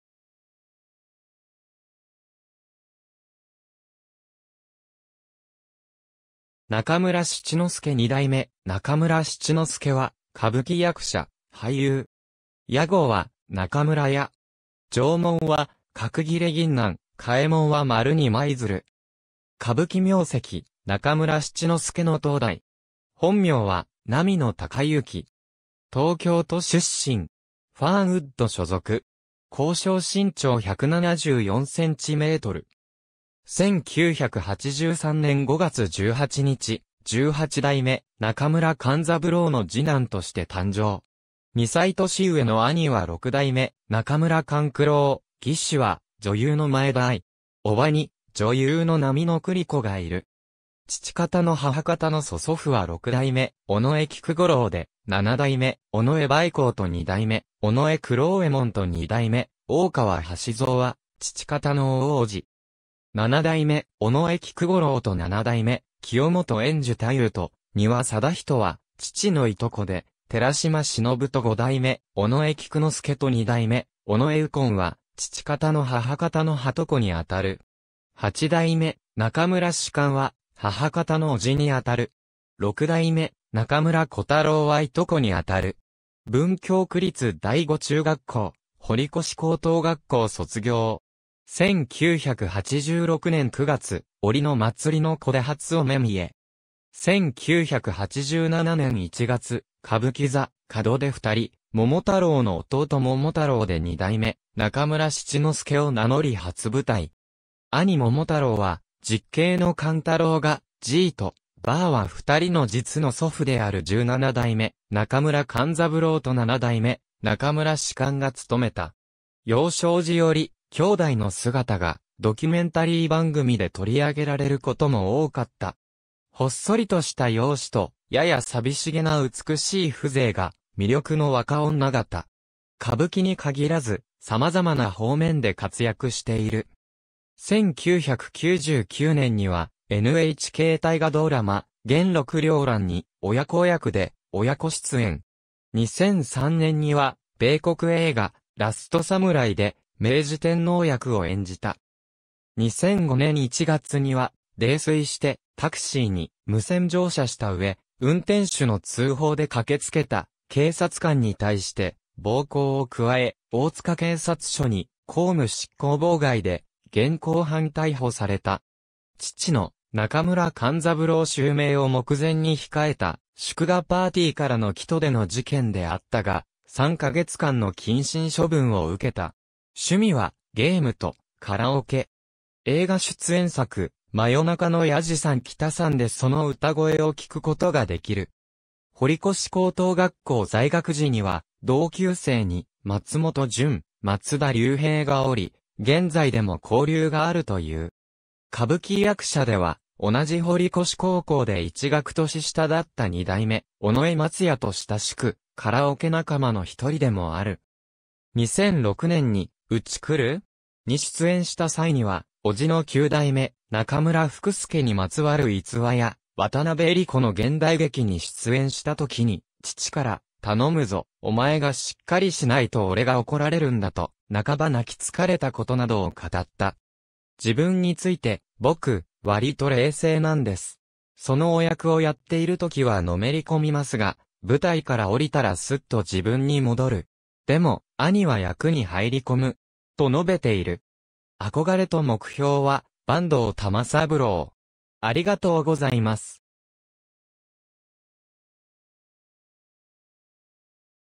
中村七之助 (2代目)
中村七之助 (2代目) 二代目 中村 七之助（なかむら しちのすけ、1983年5月18日 - ）は、歌舞伎役者、俳優。
屋号は中村屋。
定紋は角切銀杏、替紋は丸に舞鶴。
歌舞伎名跡「中村七之助」の当代。
本名は波野 隆行（なみの たかゆき）。
東京都出身。
ファーンウッド所属。
公称身長174cm。
1983年（昭和58年）5月18日、十八代目中村勘三郎（当時は五代目中村勘九郎）の次男として誕生。
2歳年上の兄は六代目中村勘九郎、義姉は女優の前田愛。
伯母に女優の波乃久里子がいる。
父方の母方の曽祖父は六代目尾上菊五郎で、七代目尾上梅幸と二代目尾上九朗右衛門と二代目大川橋蔵は父方の大伯父（大叔父）。
七代目尾上菊五郎と七代目清元延寿太夫と丹羽貞仁は父（及び伯母）の従兄弟で、寺島しのぶと五代目尾上菊之助と二代目尾上右近は父方の母方の再従兄弟にあたる（お互いの祖父母同士が兄弟のため）。
八代目中村芝翫は母方の叔父にあたる。
六代目中村児太郎は従兄弟にあたる。
文京区立第五中学校、堀越高等学校卒業。
1986年（昭和61年）9月、『檻』の祭りの子で初お目見得。
1987年（昭和62年）1月、歌舞伎座『門出二人桃太郎』の弟桃太郎で二代目中村七之助を名乗り初舞台。
兄桃太郎は実兄の勘太郎(現:勘九郎)が、爺と婆は二人の実の祖父である十七代目中村勘三郎（爺）と七代目中村芝翫（婆）がつとめた。
幼少時より、兄弟の姿がドキュメンタリー番組で取り上げられることも多かった。
ほっそりとした容姿と、やや寂しげな美しい風情が魅力の若女方。
歌舞伎に限らず、様々な方面で活躍している。
1999年（平成11年）には、NHK大河ドラマ『元禄繚乱』に親子役で親子出演（父が大石内蔵助、本人が大石主税）。
2003年（平成15年）には米国映画『ラスト サムライ』で明治天皇役を演じた。
2005年（平成17年）1月には、泥酔してタクシーに無銭乗車したうえ、運転手の通報で駆けつけた警察官に対して暴行を加え、大塚警察署に公務執行妨害で現行犯逮捕された。
父の「中村勘三郎」襲名を目前に控えた祝賀パーティーからの帰途での事件であったが、3ヵ月間の謹慎処分を受けた。
趣味はゲームとカラオケ。
映画出演作『真夜中の弥次さん喜多さん』でその歌声を聞くことができる。
堀越高等学校在学時には同級生に松本潤、松田龍平がおり、現在でも交流があるという。
歌舞伎役者では、同じ堀越高校で1学年下だった二代目尾上松也と親しく、カラオケ仲間のひとりでもある。
2006年に『ウチくる!?』に出演した際には、叔父の九代目中村福助にまつわる逸話や、渡辺えり子の現代劇に出演したときに父から「頼むぞ、お前がしっかりしないと俺が怒られるんだ」と半ば泣きつかれたことなどを語った。
自分について、「僕、割と冷静なんです。そのお役をやっているときはのめりこみますが、舞台から下りたらスッと自分に戻る。でも、兄（勘九郎）は役に入り込む。」と述べている。
憧れと目標は、坂東玉三郎。